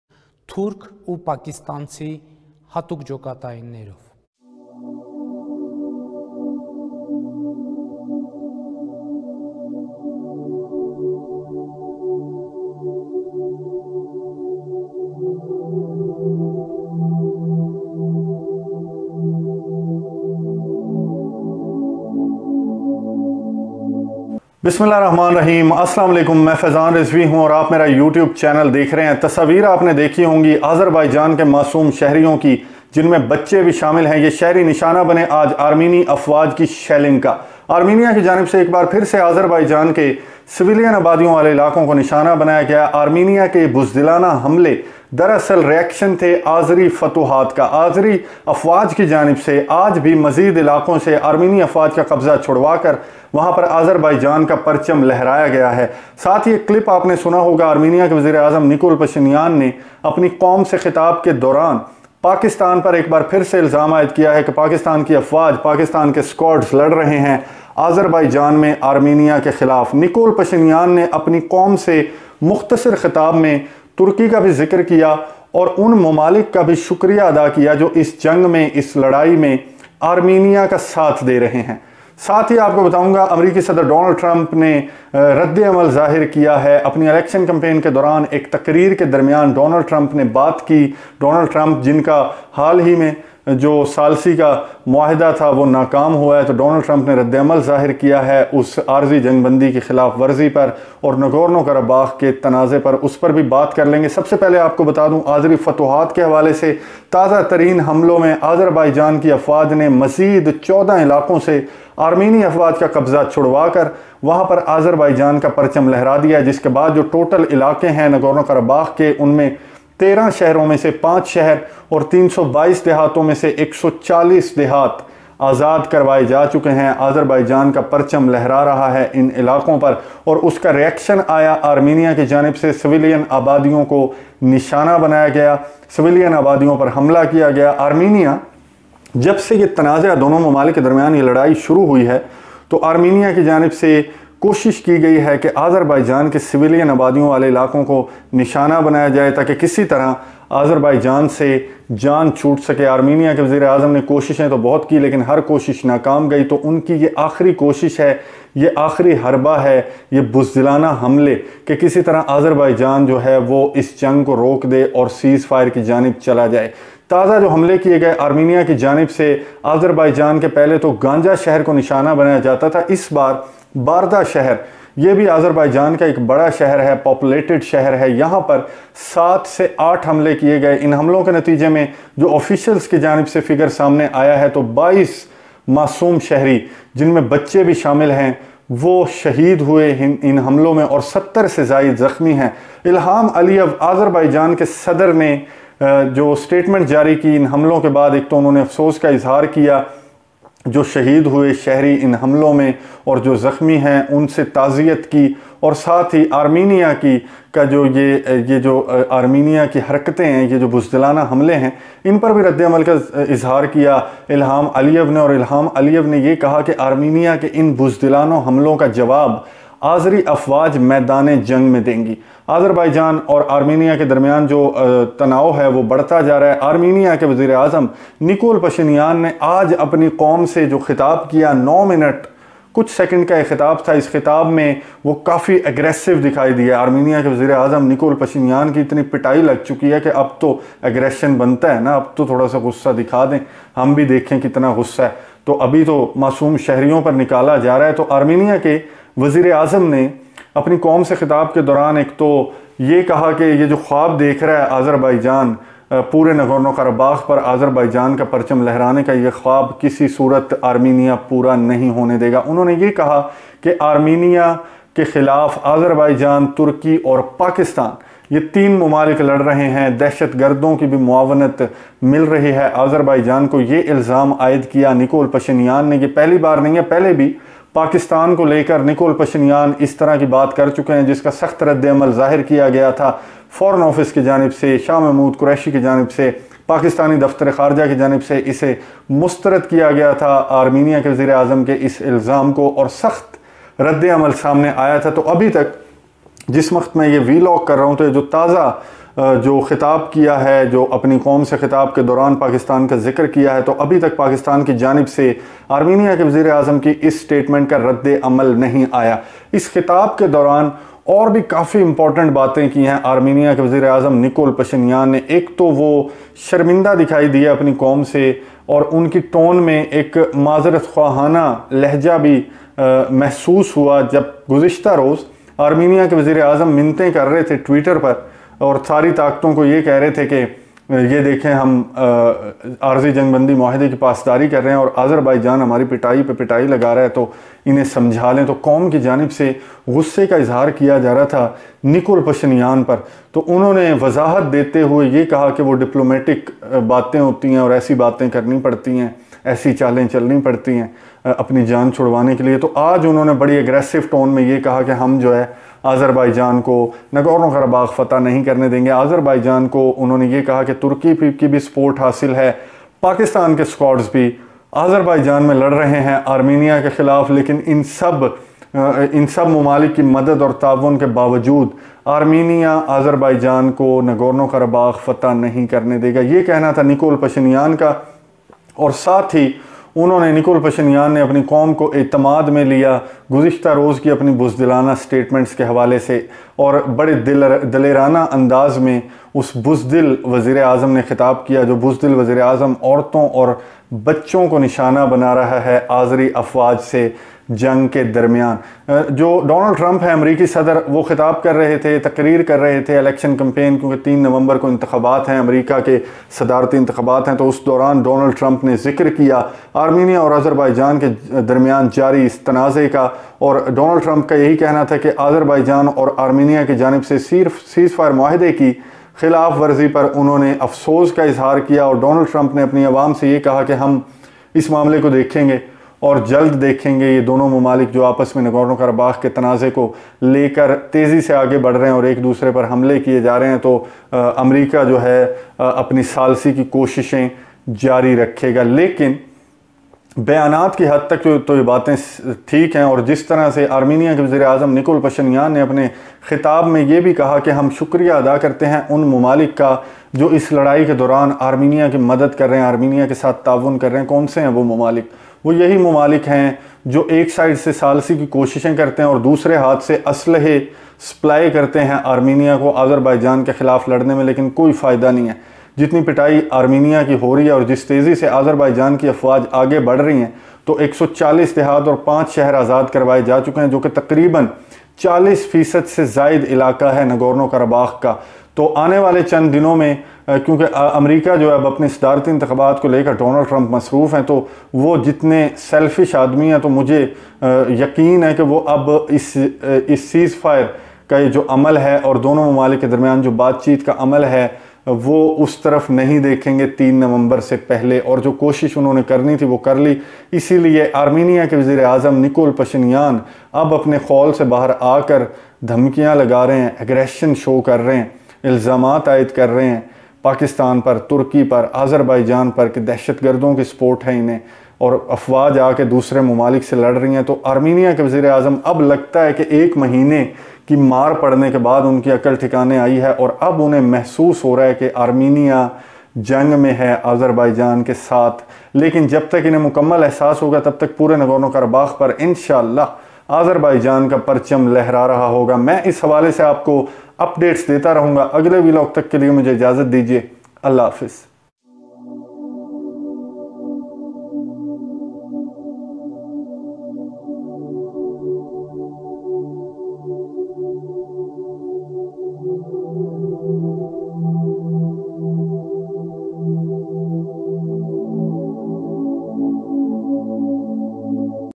Nagorno-Karabakh || Armenia's Allegations || Azerbaijan's Reply || Faizan Rizvi
Video is about the latest situation in Nagorno-Karabakh between Azerbaijan and Armenia.